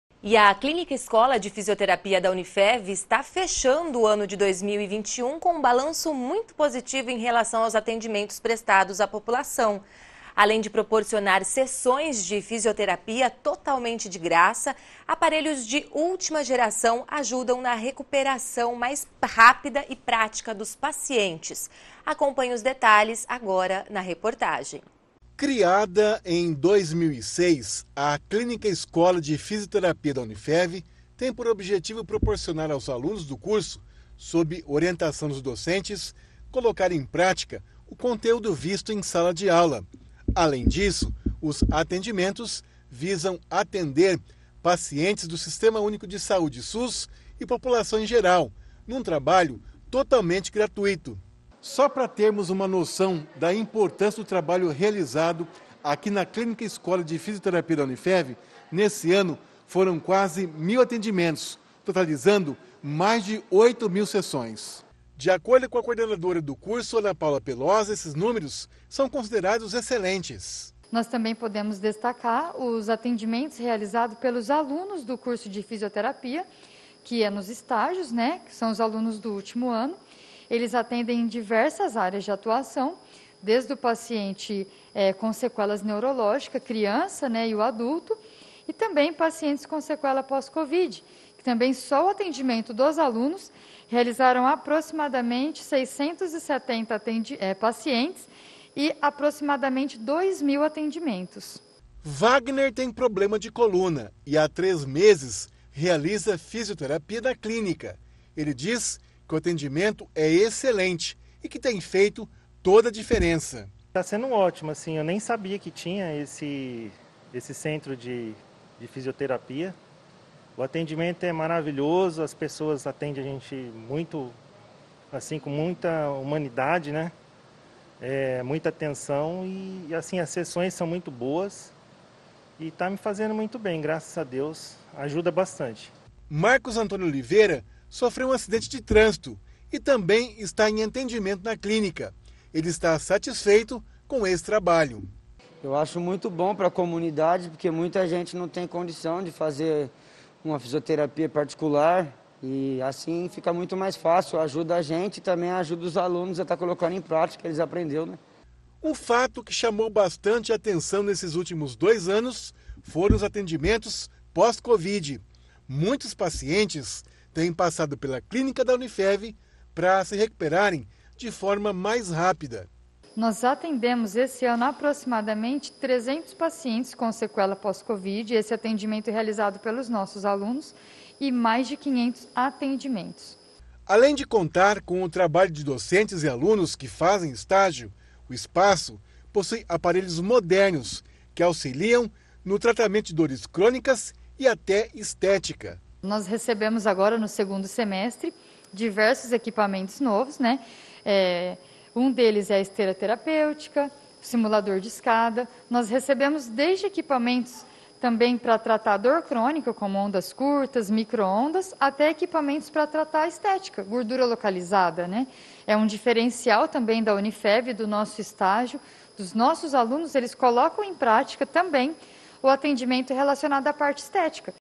Balanço da Fisioterapia
A clínica escola de fisioterapia da Unifev está fechando o ano de 2021 com um balanço muito positivo em relação aos atendimentos prestados à população. Além de proporcionar sessões de fisioterapia totalmente de graça, aparelhos de última geração ajudam na recuperação mais rápida dos pacientes.